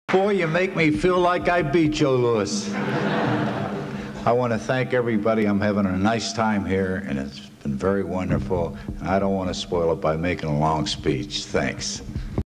Dick Conlon introduced by Billy Conn
Billy Conn introduces inductee Dick Conlon at the 1988 Blair County Sports Hall of Fame